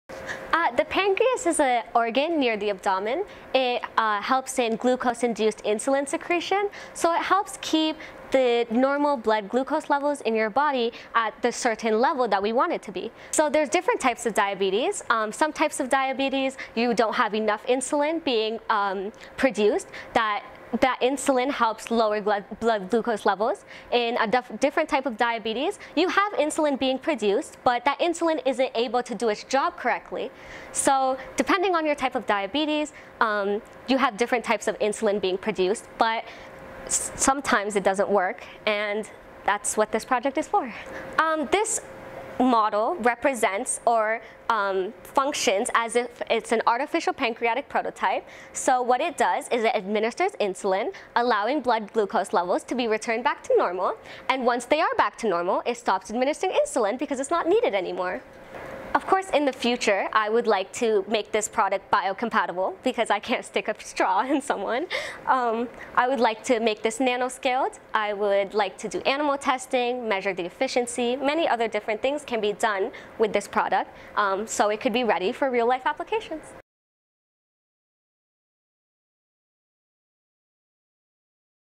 Assumption Student Creates Artificial Pancreas
Please join us in wishing Sohila Sidhu good luck as she prepares for the Canada-Wide Science Fair, being held virtually next week from Fredericton, New Brunswick. A Gr 10 student at Assumption College Catholic High School, she developed a prototype for an artificial pancreas that could help people living with diabetes, a project which won her the University of Windsor Faculty of Science Award at the 59th annual Windsor Regional Science Technology and Engineering Fair.